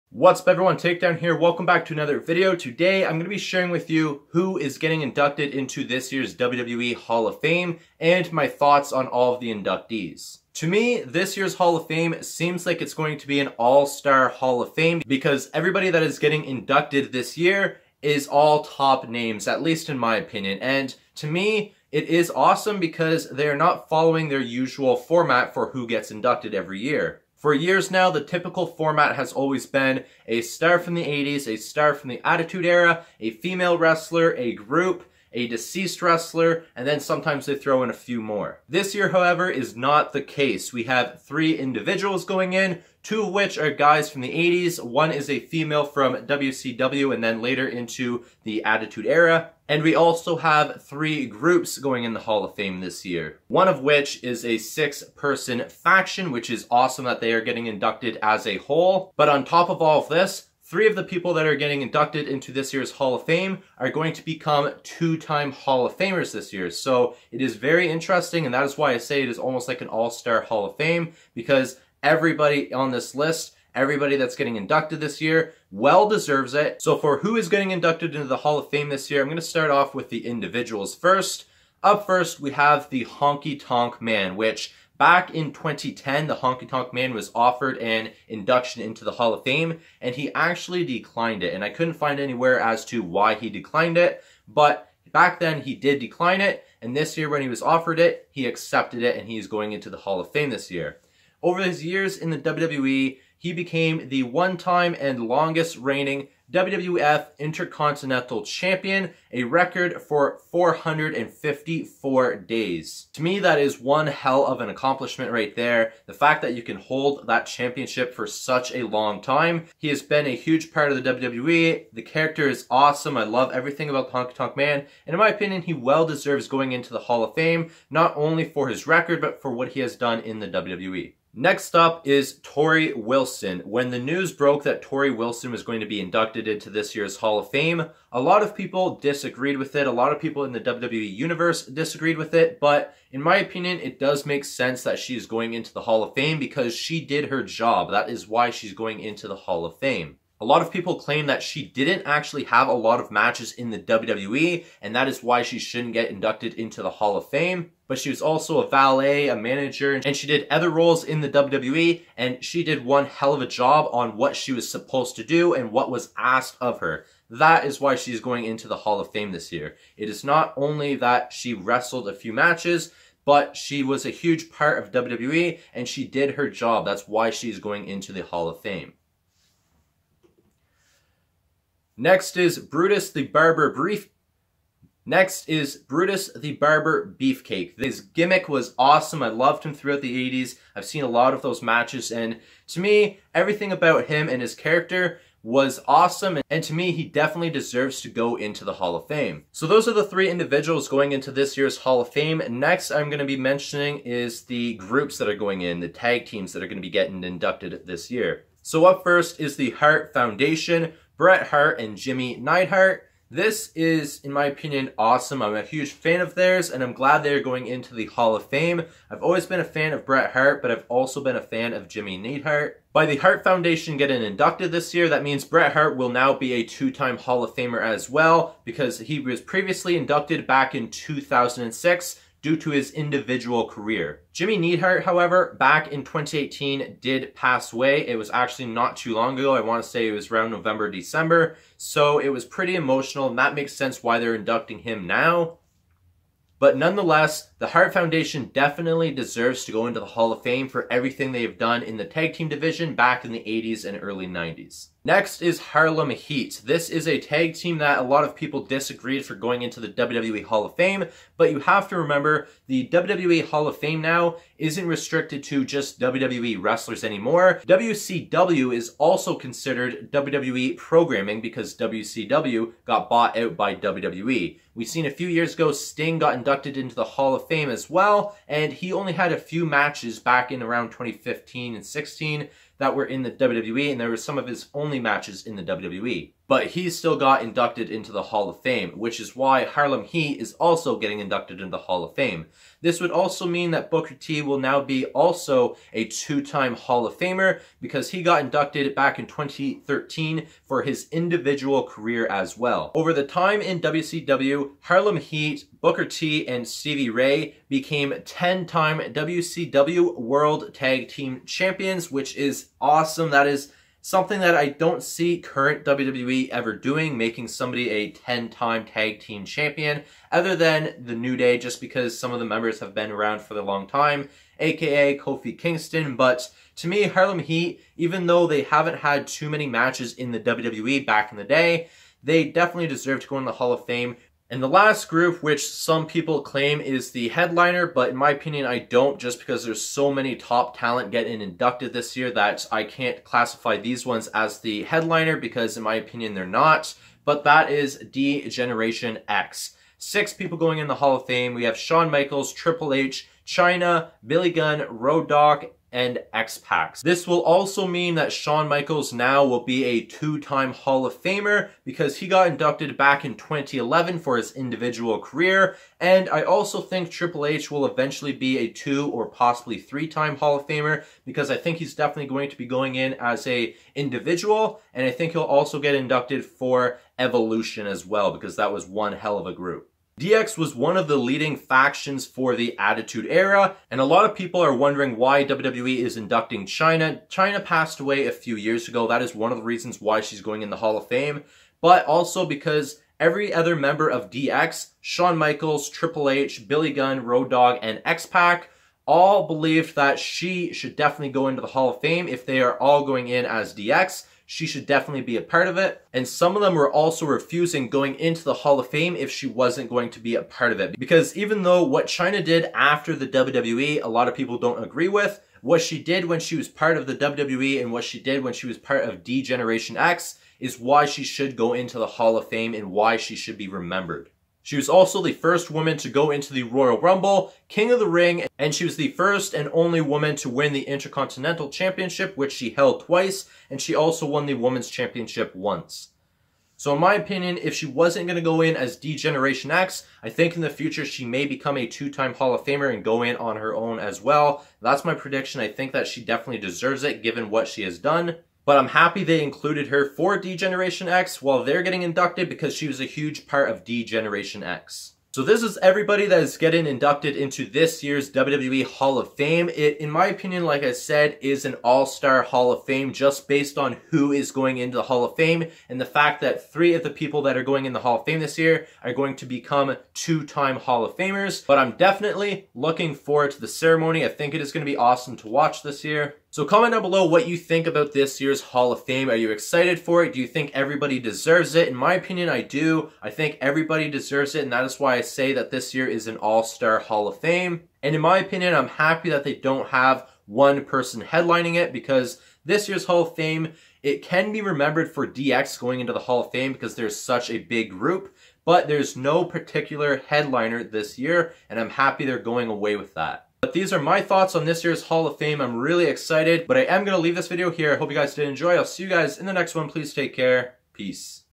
WWE HALL OF FAME CLASS OF 2019
What are your opinions on the 2019 WWE Hall of Fame Inductee's? To me it seems like this year is almost like an All-Star Hall of Fame based on the ones getting inducted are top names in my opinion